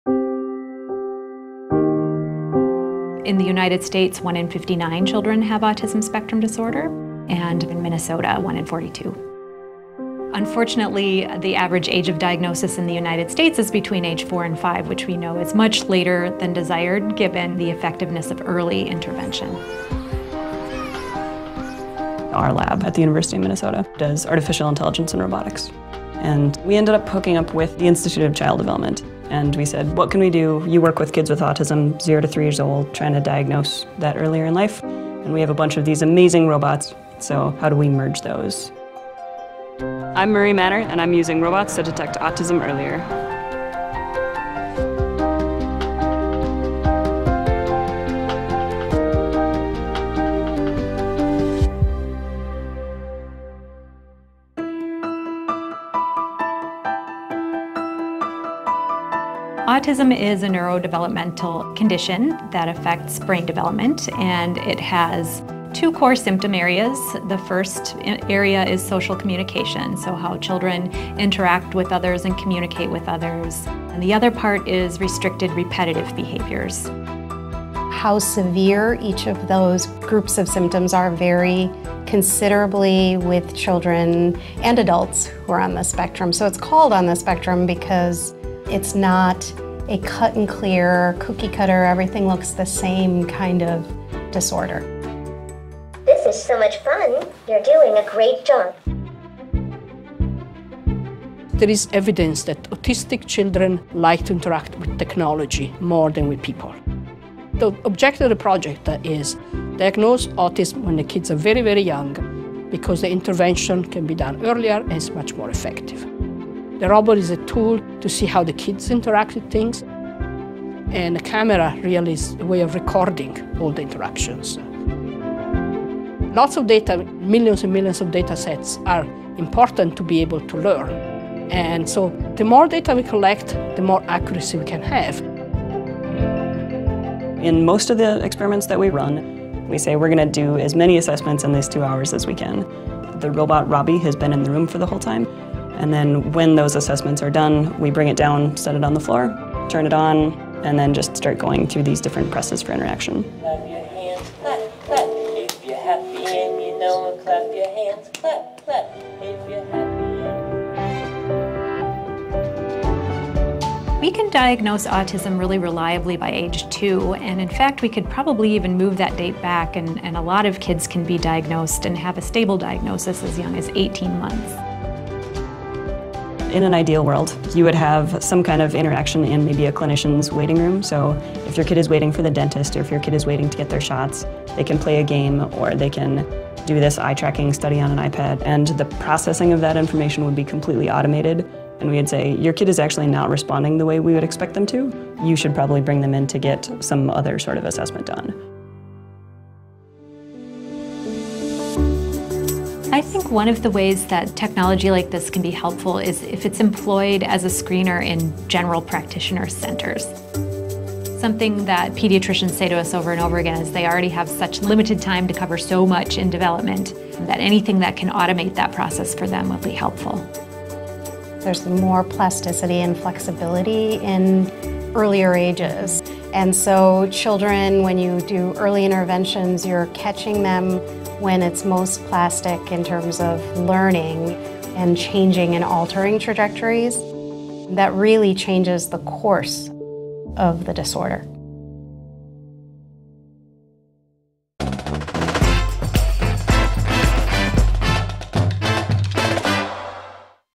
Unlocking Mechanical Minds: using robots to help diagnose autism
Kids’ interactions with talking robots—which behave in a standardized, bias-free manner—could reveal telltale signs in very young children destined to develop autism. University of Minnesota researchers have collected baseline data on robots’ interactions with 2- to 4-year-olds. They hope to use this data to predict who will develop autism, unlocking the door to countless precious minds.